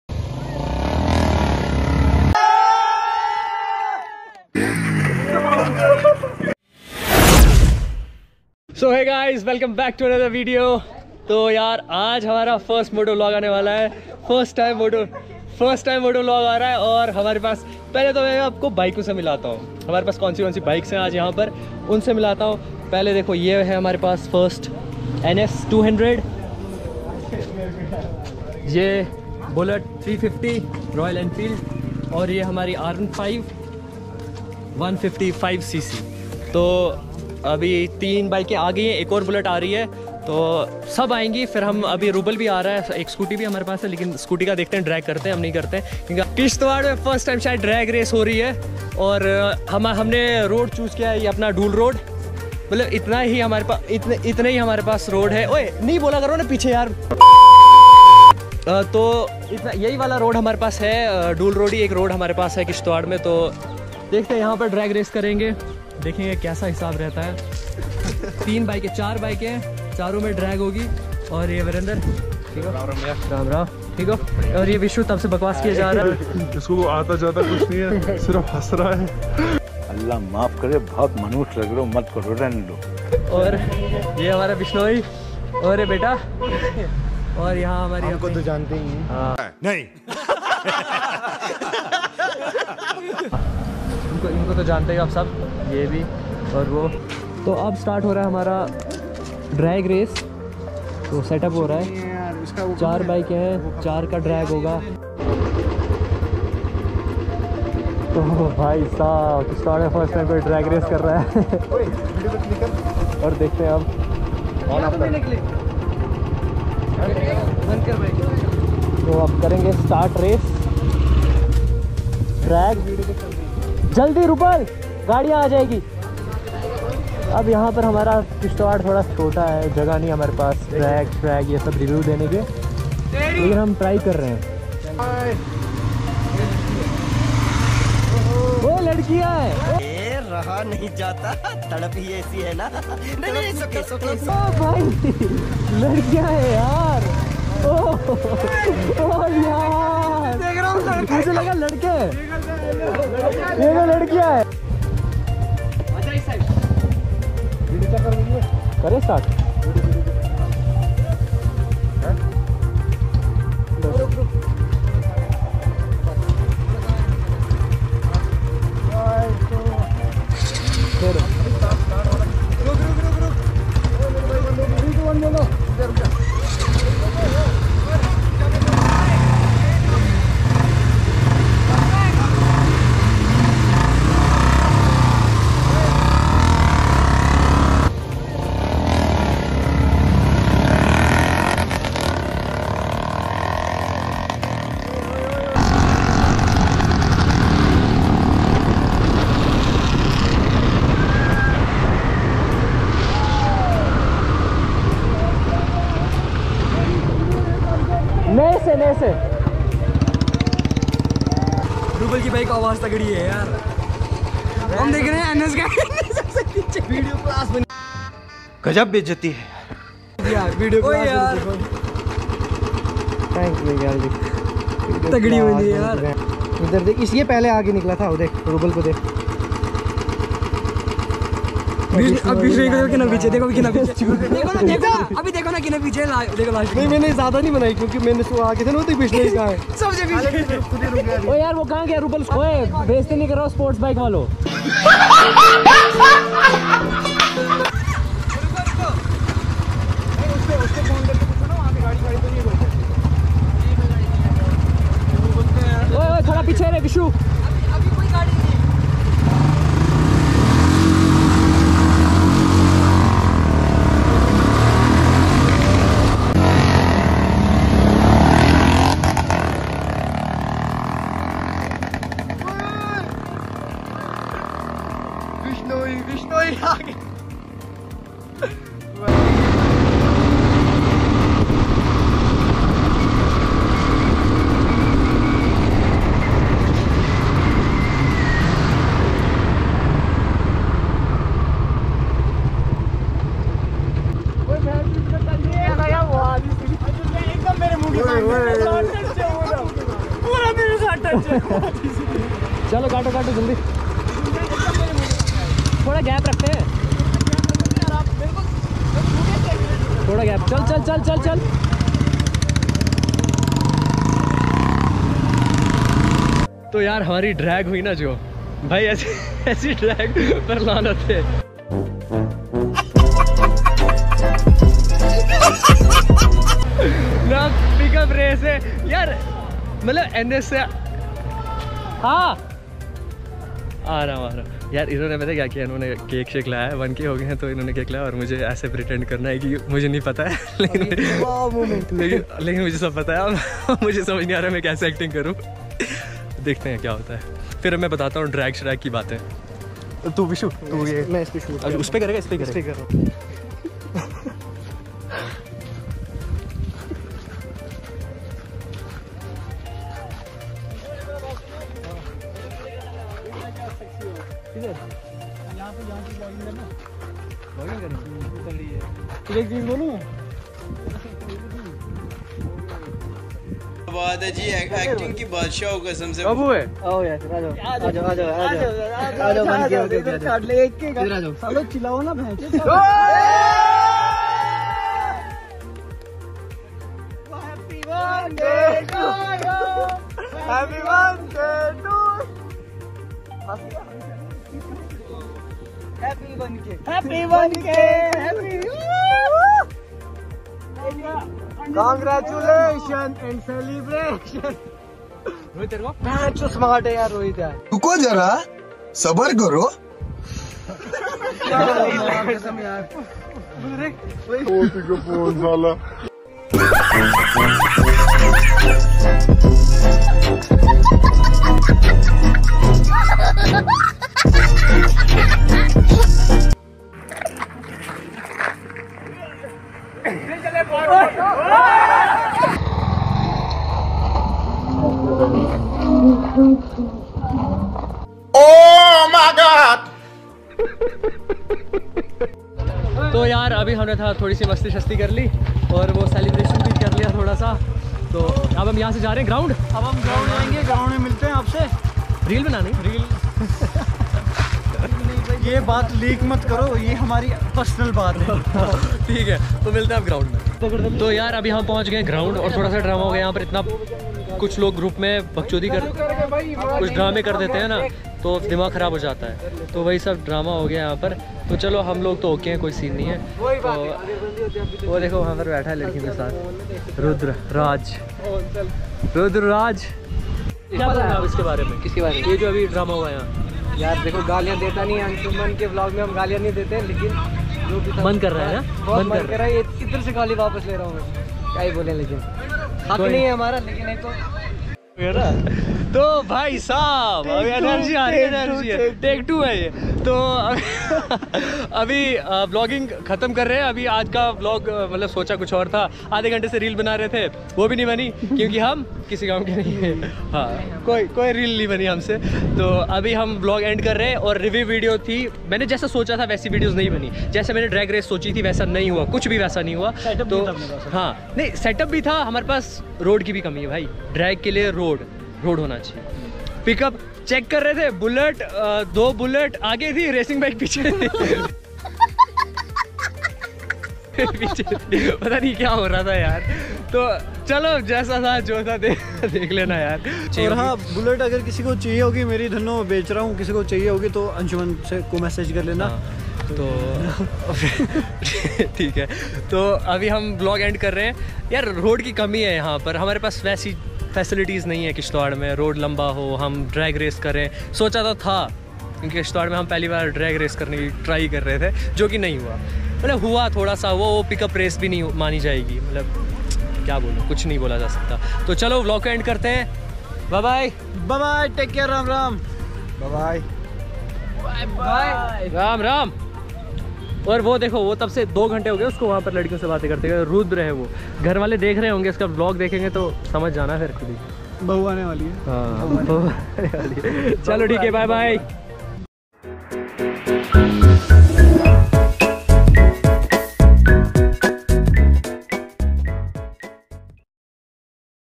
Drag race in kishtwar//Vlog : BULLET 350🔥vs r15🔥vs NS 200🔥vs BULLET 350🔥😱1k celebration
follow me -